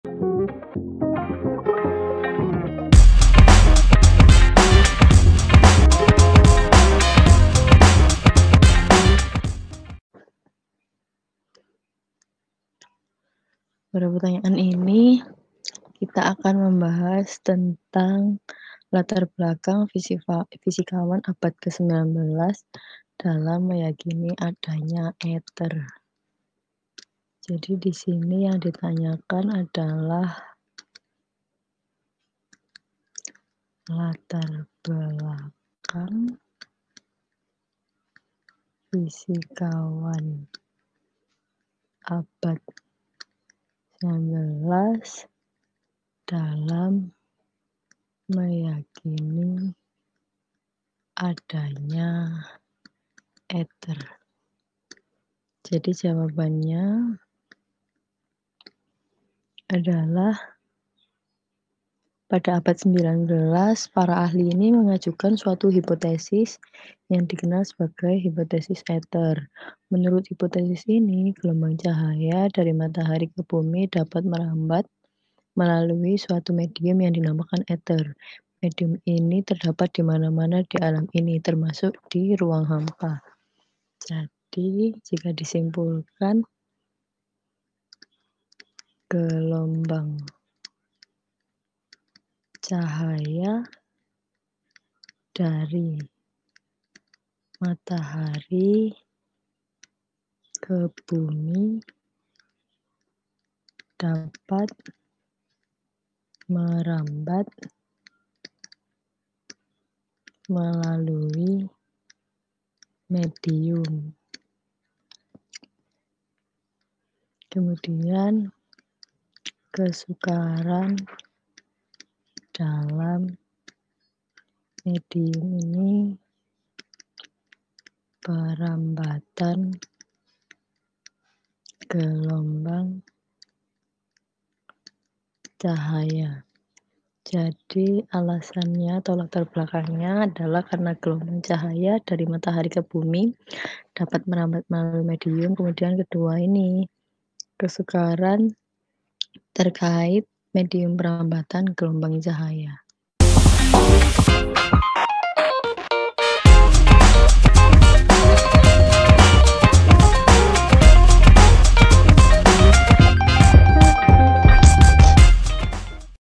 Sebutkan latar belakang para fisikawan aba... | Mudah dan Aktif | Kelas 12 | 9 | Fisika
✅ Dalam video ini kita akan membahas: Sebutkan latar belakang para fisikawan abad ke-19 dalam meyakini adanya eter!
Tema:
Kelas: 12
Mata Pelajaran: Fisika
Bab: 9
Buku: Mudah dan Aktif Belajar Fisika Untuk Kelas XII/3

👉 Buku yang telah tersedia:
{PLAYLIST_LINK}
Di mana kamu dapat menemukan kami?